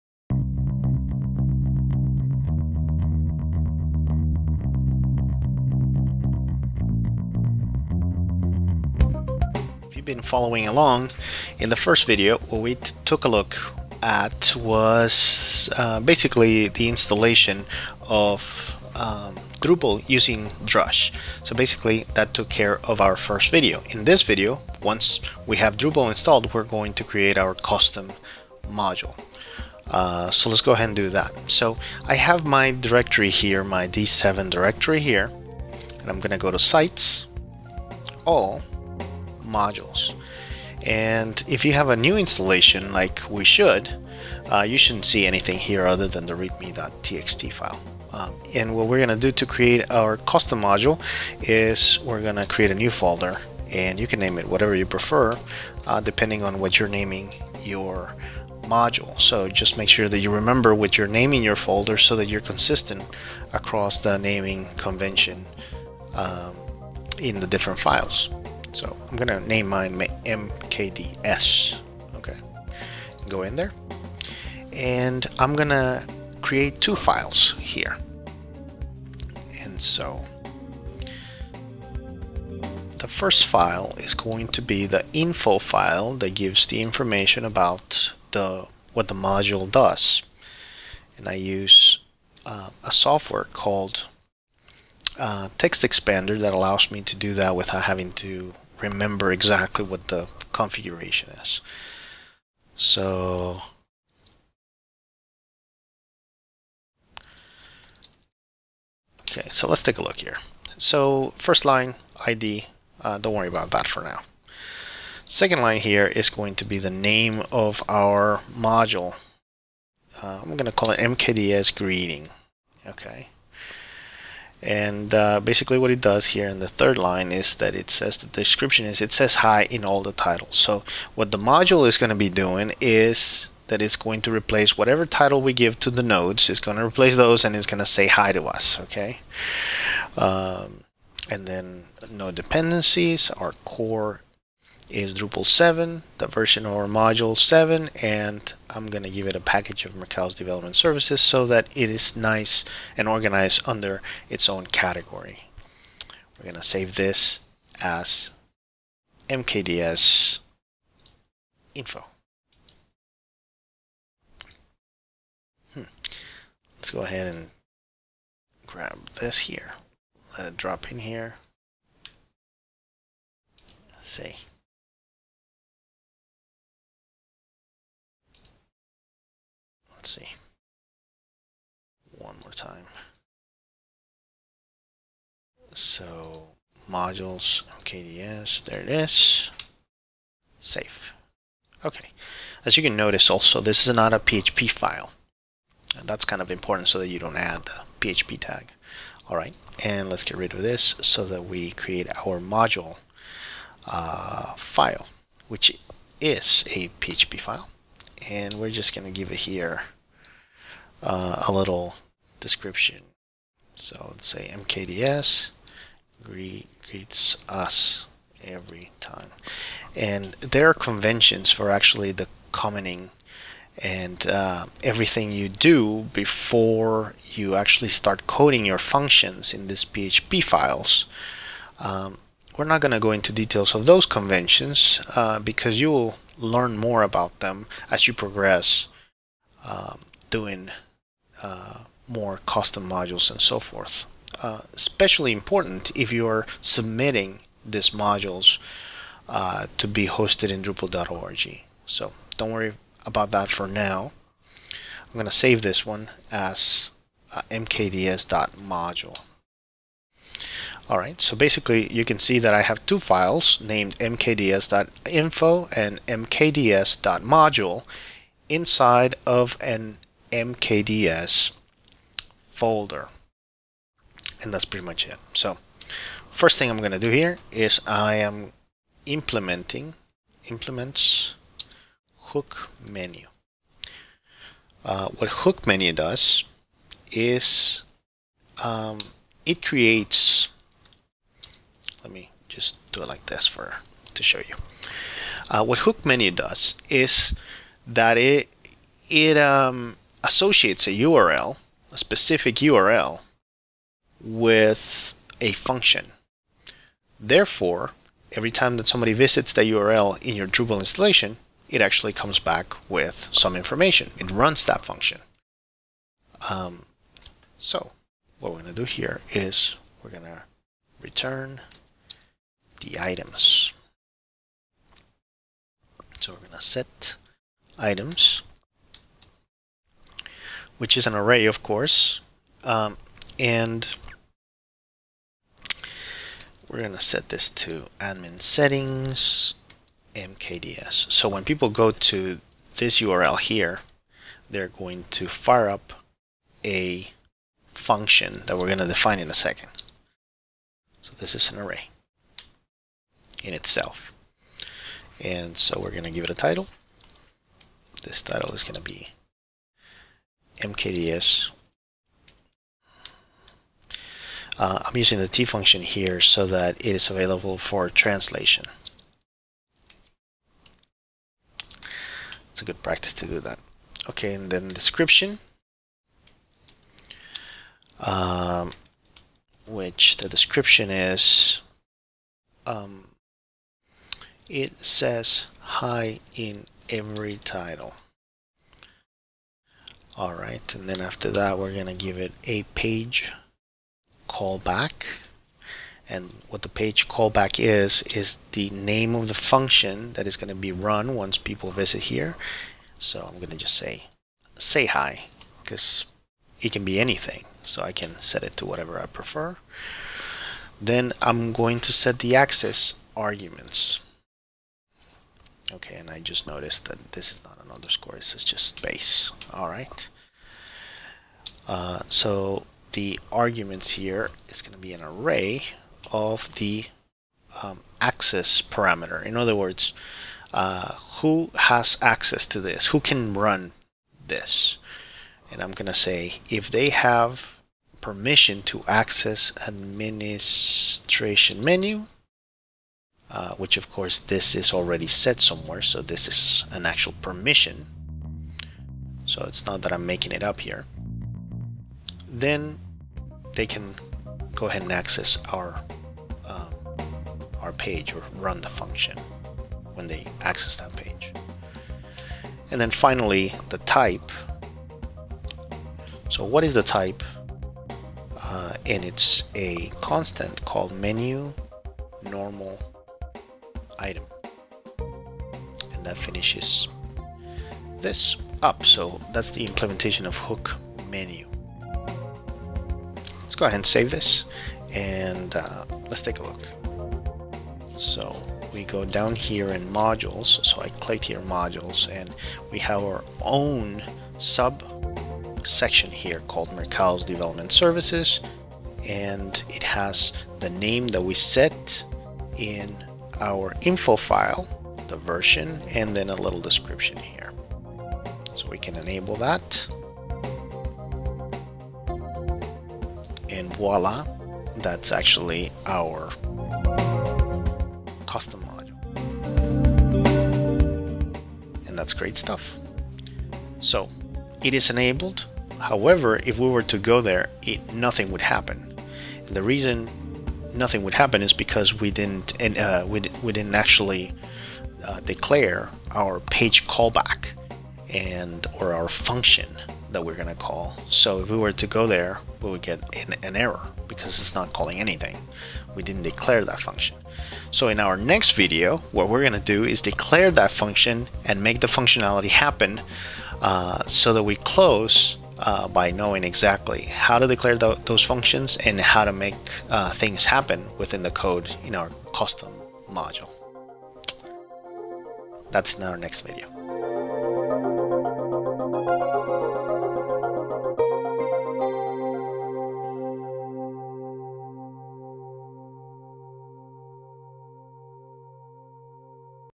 Developing Custom Drupal Modules Part 2: Create the Module
In this episode, Alex Centeno, creative director at Merkados, shares how to start your module development, create the proper folder and files necessary. This video is part of a series where we'll learn how to create our own custom modules.